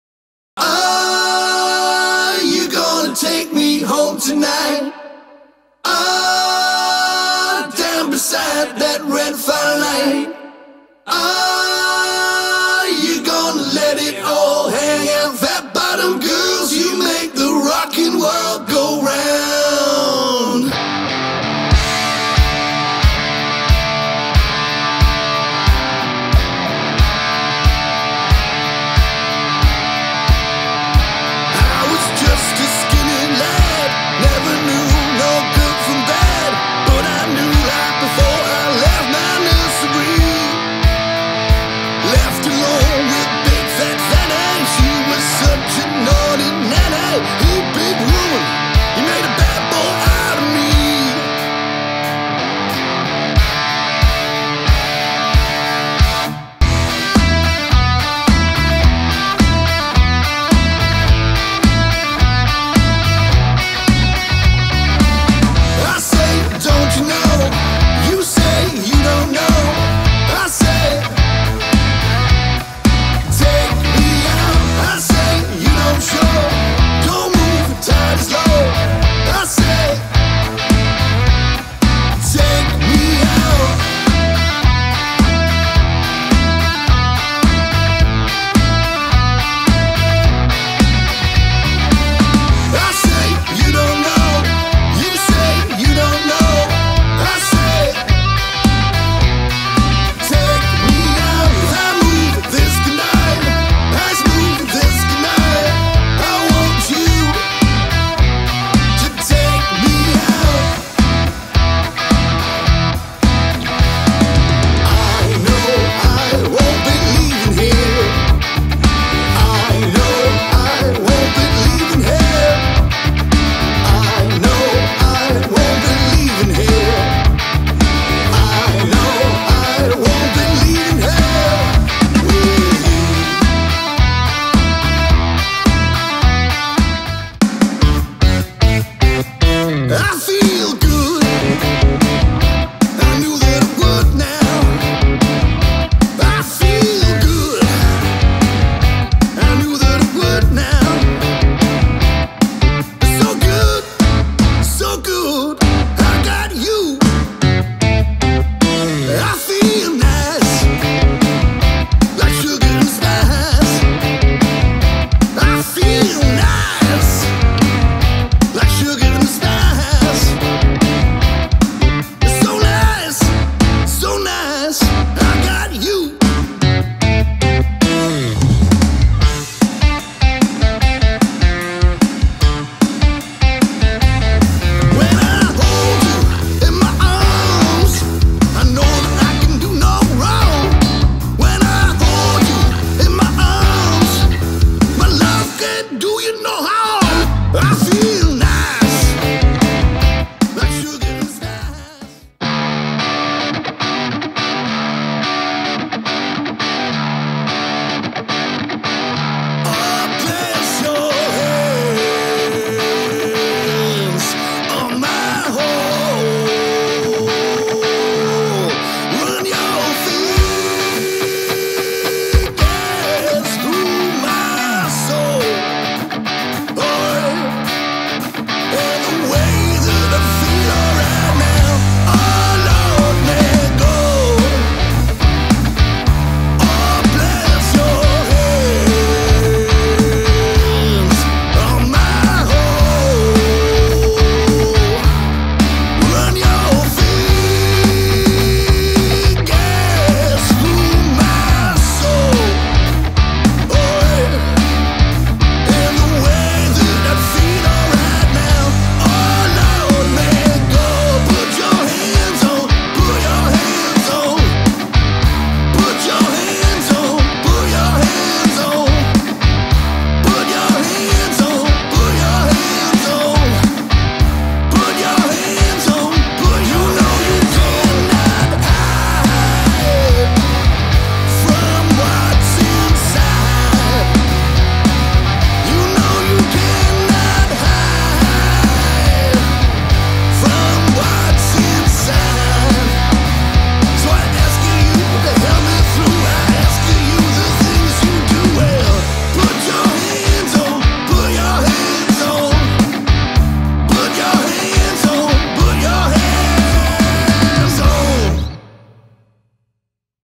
Rock and Pop Party Band The 123s - AliveNetwork.com
Playing banging songs that’ll keep you dancing & singing all night long, The MP3's deliver infectious personality, huge talent & a powerful party vibe! Covering much-loved guitar anthems, from modern Indie to classic Rock & retro Rock n' Roll, The 123's are not to be missed!